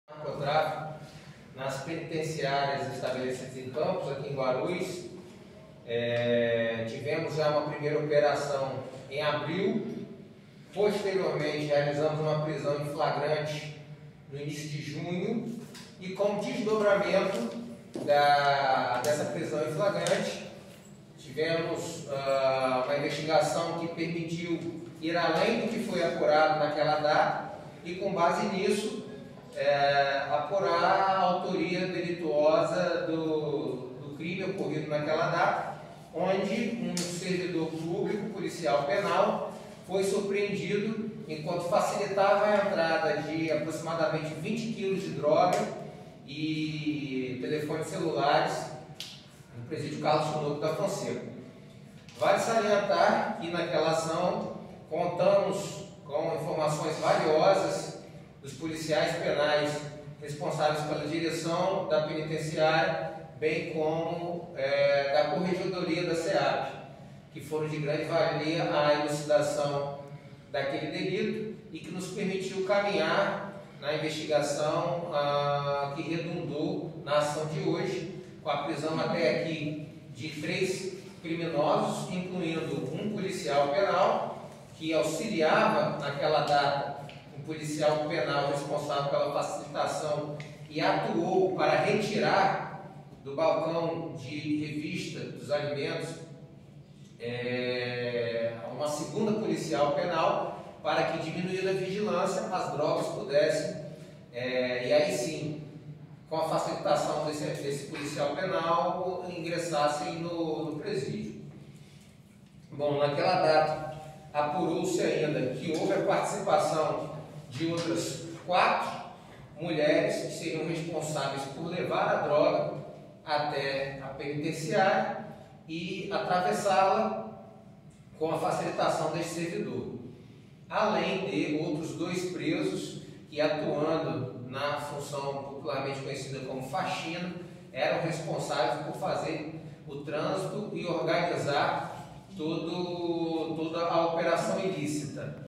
Policial penal preso em operação em Campos; Delegado fala sobre ação
Três pessoas foram presas, incluíndo um policial penal, na manhã desta quinta-feira (22), durante a Operação Simetria II em Guarus. Outros dois alvos da operação estão sendo procurados.

A Operação visa combater o tráfico de drogas dentro dos estabelecimentos prisionais de Campos e conta com o apoio do 6º Comando de Policiamento de Área, com o 8º Batalhão de Polícia Militar e com a 2ª Seção do Oitavo Batalhão.
(Vídeo- NF Notícias)